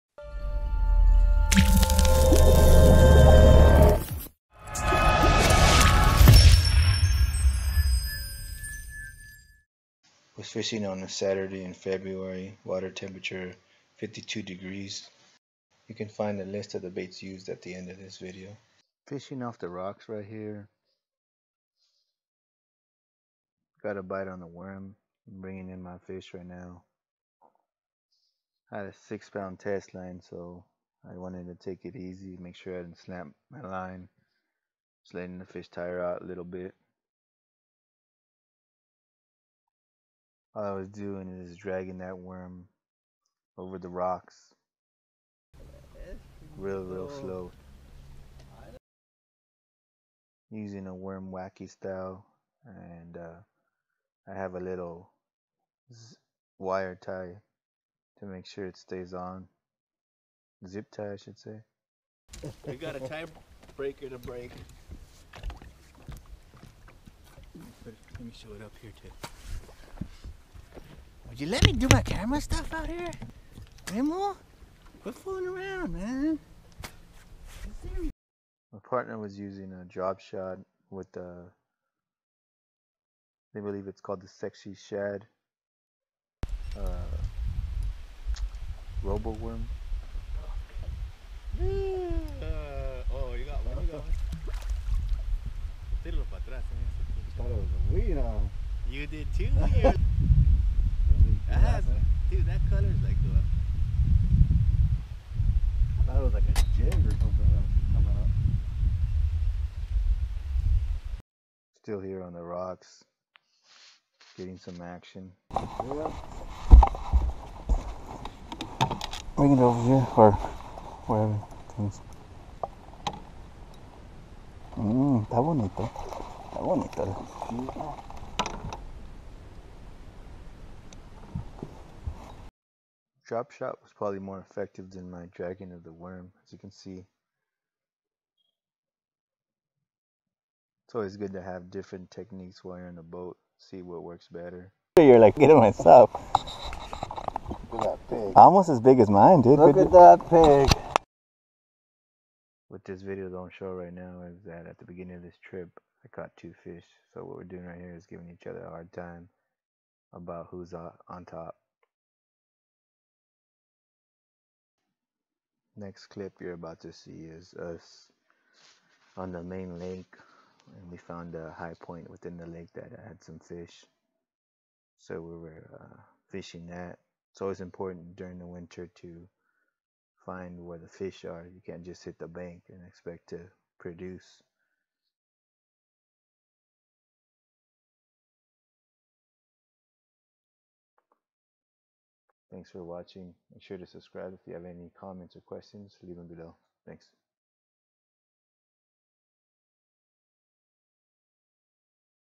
Bass Fishing - Finesse Fishing During the Winter (Millerton Lake, California)
Winter Bass fishing trip on 2/4/17 to Millerton Lake in California. finesse  fishing with worms, dragging a worm and drop-shotting.

Lures used:
- Zoom WormGreen Pumpkin Magic
- Roboworm Hologram shad

G’s-Up Fishing